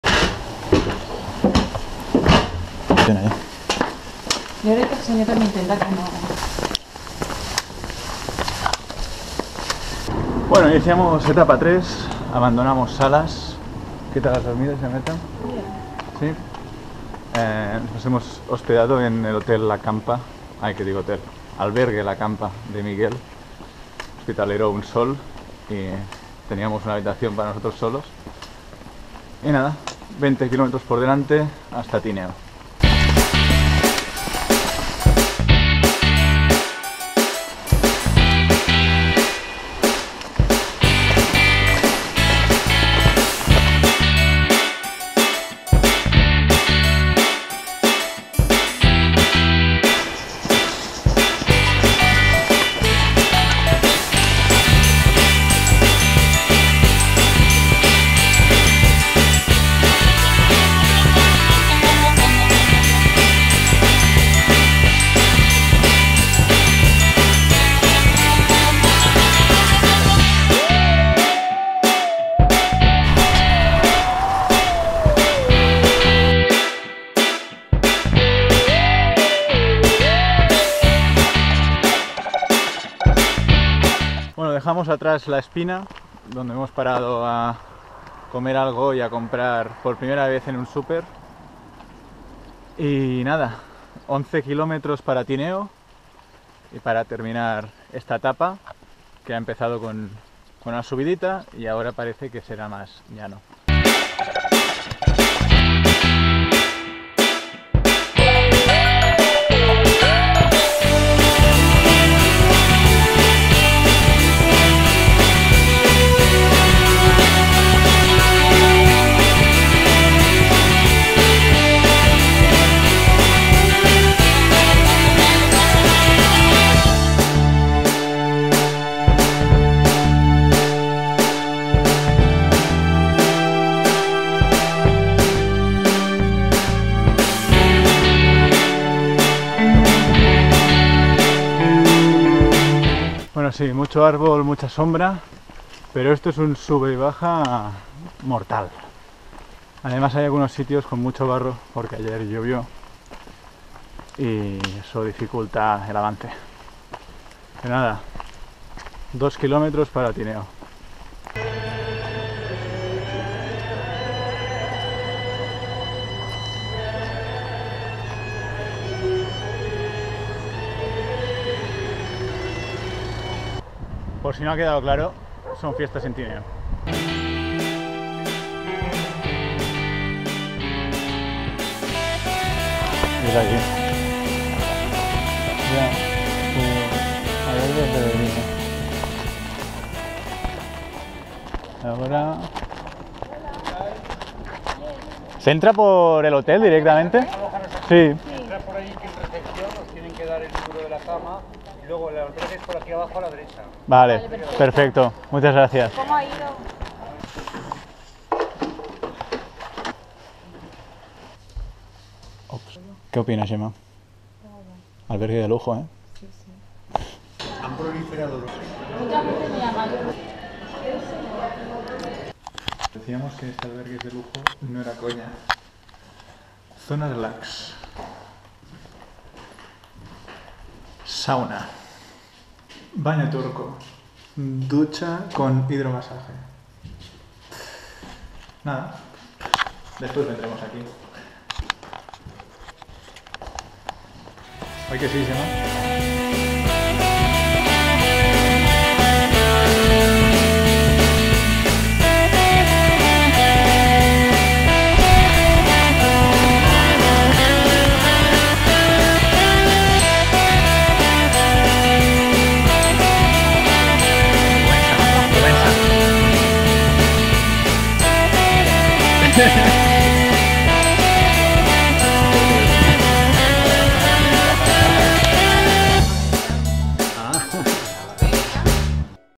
Camino de Santiago Primitivo | Etapa 3 | Salas - Tineo
Tercera etapa, cruzamos bosques y prados para llegar a Tineo, donde nos aguardan lujos inesperados.